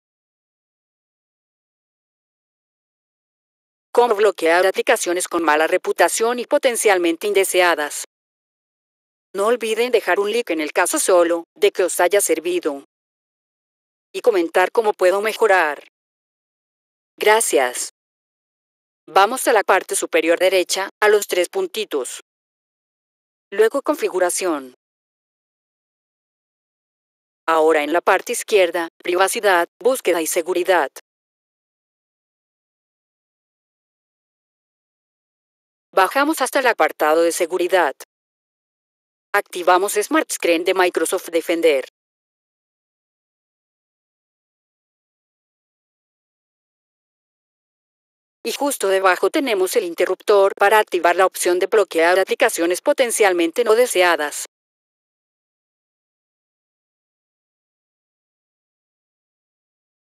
Bloquear Aplicaciones Indeseadas Edge
Cómo Bloquear aplicaciones con mala reputación y potencialmente indeseadas.
No olviden dejar un like en el caso solo, de que os haya servido. Y comentar cómo puedo mejorar. Gracias.
Vamos a la parte superior derecha, A los tres puntitos. Luego configuración.
Ahora en la parte izquierda, Privacidad, búsqueda y seguridad.
Bajamos hasta el apartado de seguridad. Activamos SmartScreen de Microsoft Defender
Y justo debajo tenemos el interruptor para activar la opción de bloquear aplicaciones potencialmente no deseadas.